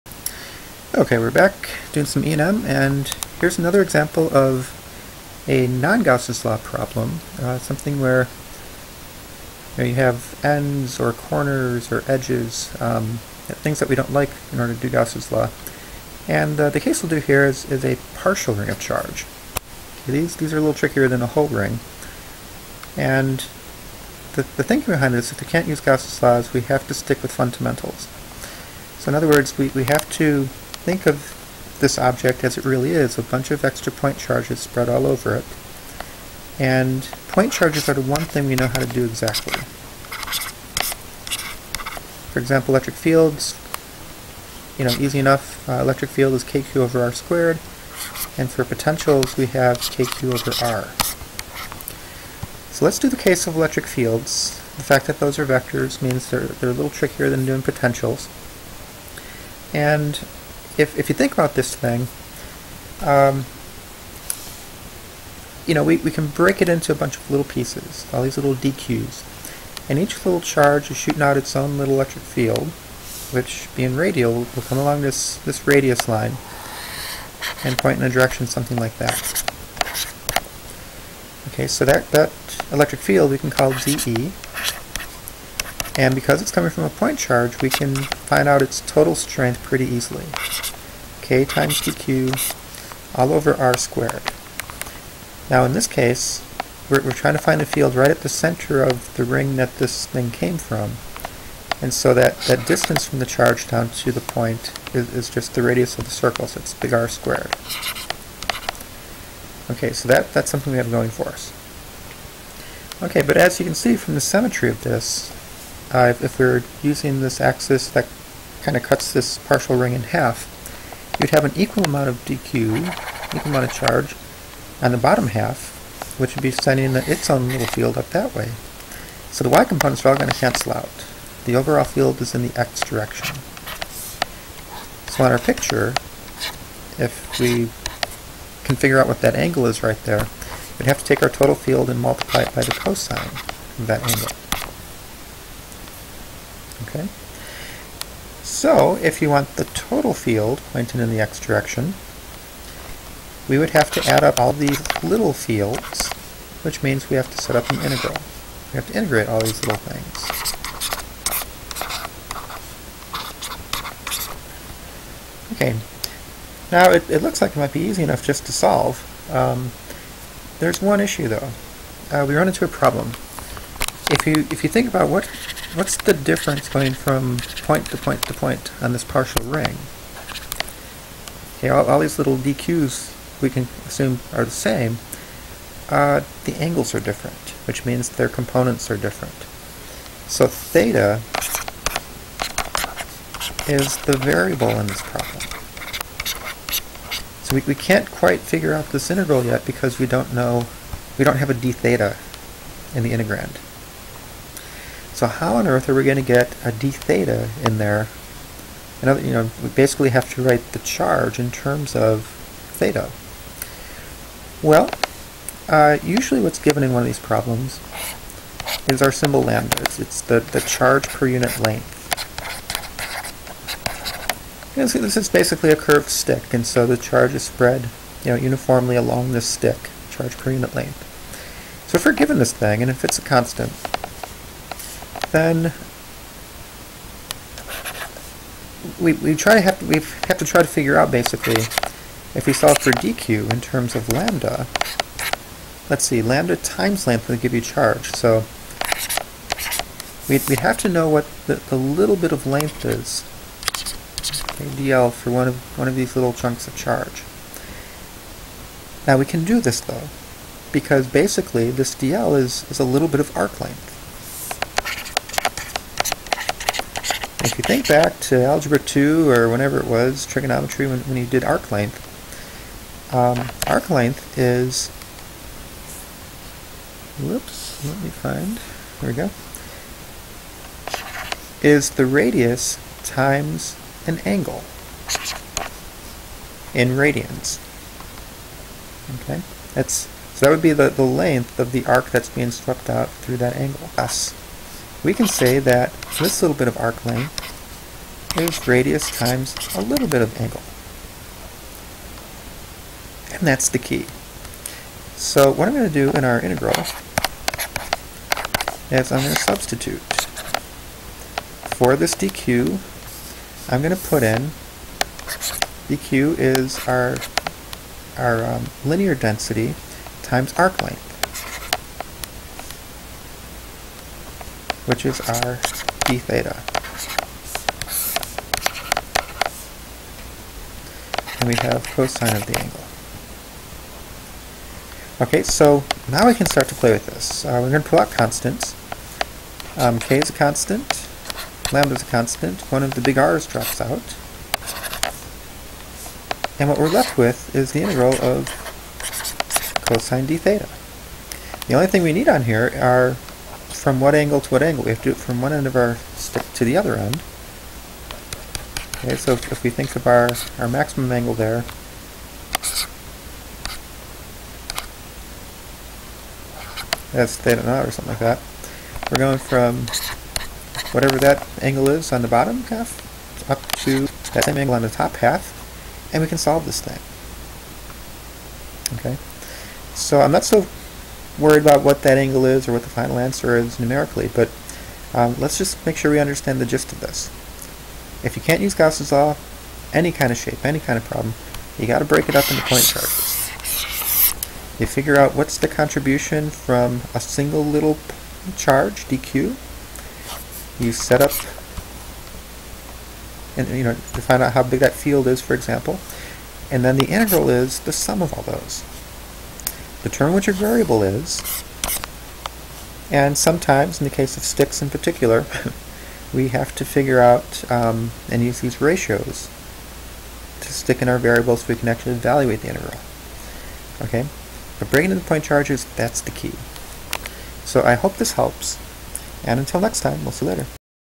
NON-Gauss's law - Partial Ring of Charge
Sometimes we get stuck with problems of finding E-fields or potentials when Gauss's law cannot be used.  A ring or partial ring would be an example.  Let's look at how to set it up for a partial ring.